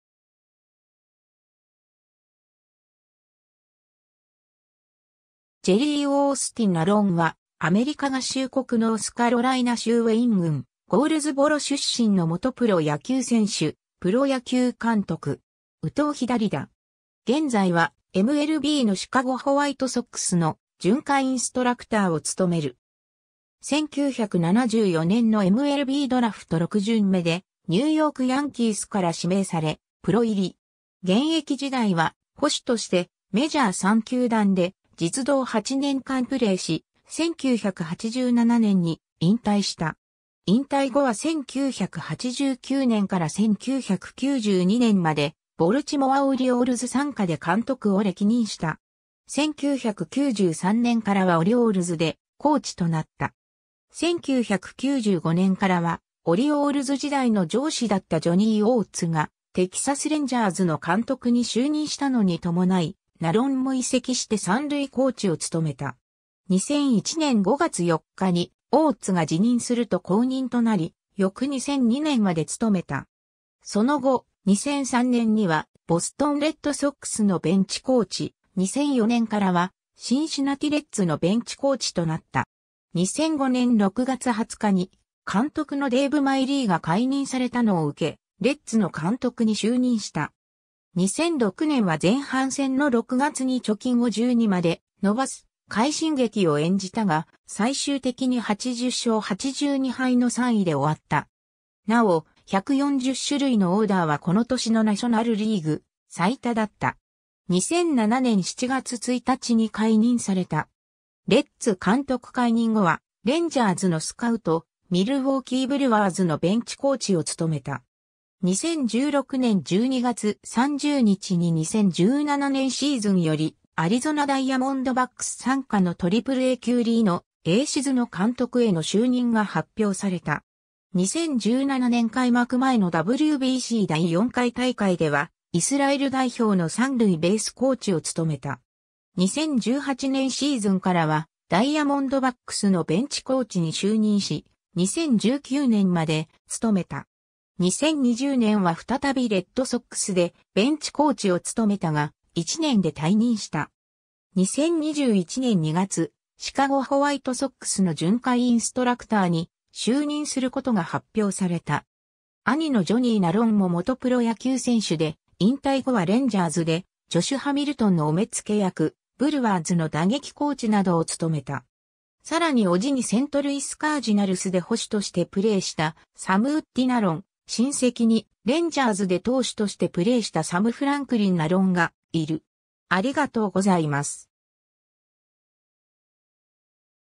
ジェリー・ナロン
シアトル・マリナーズ傘下の選手
シアトル・マリナーズの選手
カリフォルニア・エンゼルスの選手
ボルチモア・オリオールズ傘下の選手
ボルチモア・オリオールズ関連人物
テキサス・レンジャーズ関連人物
ジェリー・オースティン・ナロン（Jerry Austin Narron, 1956年1月15日 - ）は、アメリカ合衆国ノースカロライナ州ウェイン郡ゴールズボロ出身の元プロ野球選手（捕手）、プロ野球監督。
右投左打。
現在は、MLBのシカゴ・ホワイトソックスの巡回インストラクターを務める。
 1974年のMLBドラフト6巡目（全体132位）でニューヨーク・ヤンキースから指名され、プロ入り。
現役時代は捕手としてメジャー3球団で実働8年間プレーし、1987年に引退した。
 引退後は1989年から1992年までボルチモア・オリオールズ傘下で監督を歴任した。
1993年からはオリオールズでコーチ（1993年はベンチコーチ、1994年は三塁コーチ）となった。
 1995年からはオリオールズ時代の上司だったジョニー・オーツがテキサス・レンジャーズの監督に就任したのに伴い、ナロンも移籍して三塁コーチを務めた。
2001年5月4日にオーツが辞任すると後任となり、翌2002年まで務めた。
 その後、2003年にはボストン・レッドソックスのベンチコーチ、2004年からはシンシナティ・レッズのベンチコーチとなった。
2005年6月20日に監督のデーブ・マイリーが解任されたのを受け、レッズの監督に就任した。
2006年は前半戦の6月に貯金を12まで伸ばす快進撃を演じたが、最終的に80勝82敗の3位で終わった。
なお、140種類のオーダーはこの年のナショナルリーグ最多だった。
2007年7月1日に解任された。
 レッズ監督解任後はレンジャーズのスカウト（2008年から2010年まで）、ミルウォーキー・ブルワーズのベンチコーチ（2011年から2015年まで）を務めた。
 2016年12月30日に2017年シーズンよりアリゾナ・ダイヤモンドバックス傘下のAAA級リノ・エーシズの監督への就任が発表された。
 2017年開幕前のWBC第4回大会ではイスラエル代表の三塁ベースコーチを務めた。
 2018年シーズンからはダイヤモンドバックスのベンチコーチに就任し、2019年まで務めた。
 2020年は再びレッドソックスでベンチコーチを務めたが、1年で退任した。
 2021年2月、シカゴ・ホワイトソックスの巡回インストラクターに就任することが発表された。
 兄のジョニー・ナロンも元プロ野球選手（マイナー選手）で、引退後はレンジャーズでジョシュ・ハミルトンのお目付役、ブルワーズの打撃コーチなどを務めた。
さらに叔父にセントルイス・カージナルスで捕手としてプレーしたサム・ウッディ・ナロン、親戚（サム・ウッディの孫）にレンジャーズで投手としてプレーしたサム・フランクリン・ナロンがいる。